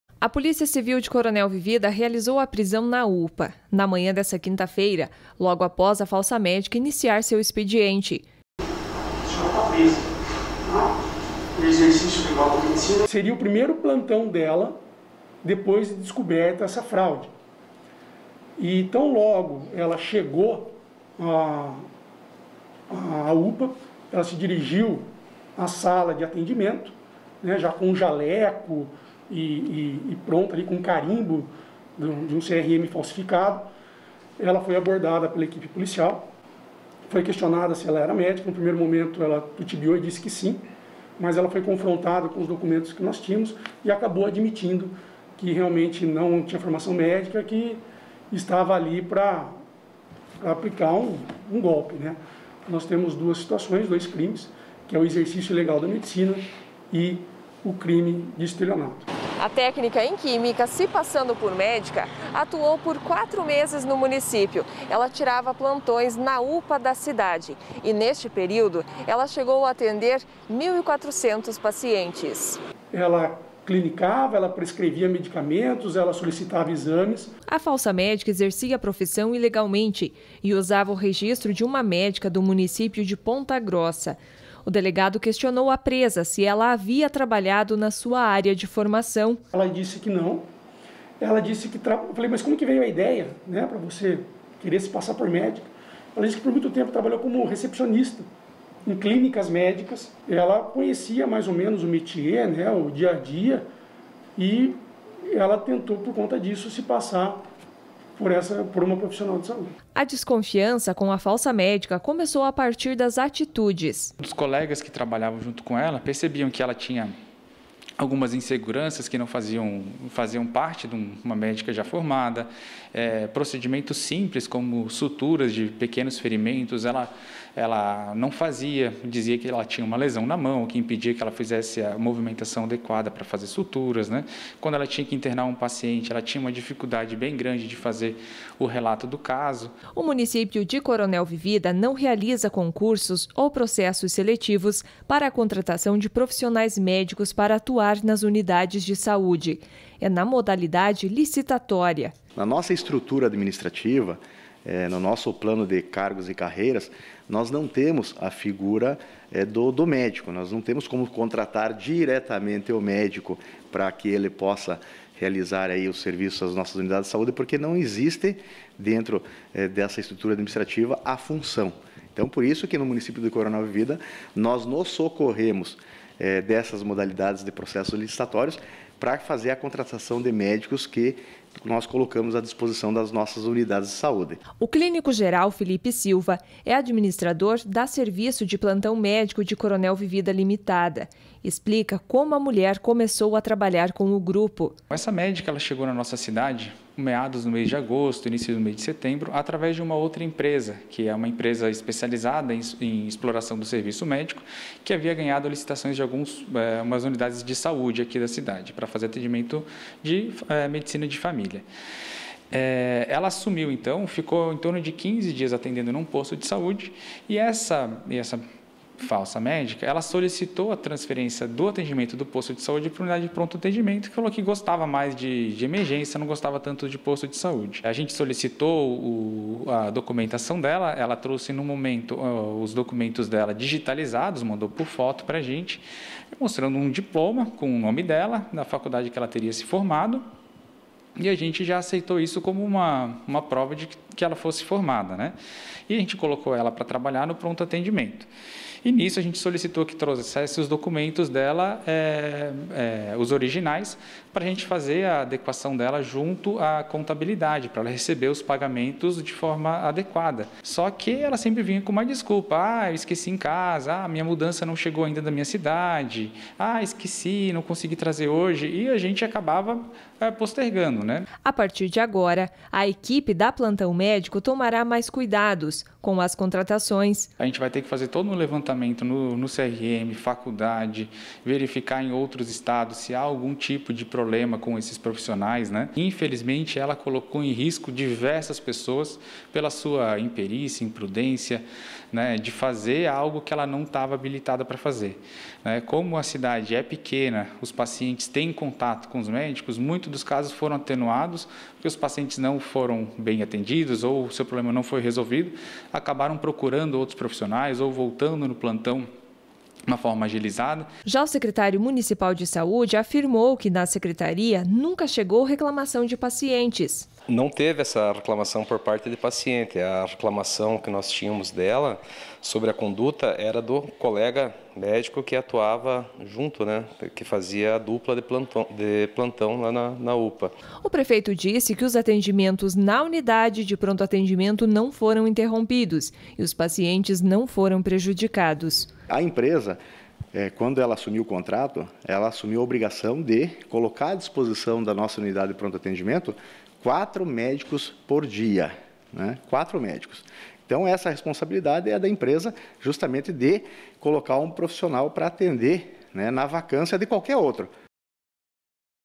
Falsa médica presa no sudoeste usava registro de uma profissional de Ponta Grossa
Segurança: Desdobramento do caso da falsa médica que atuava na UPA de Coronel Vivida. Nossa equipe conversou hoje (14/01) com o administrador do grupo que a contratou, eles contam como foi o processo até a contratação da mulher que foi presa.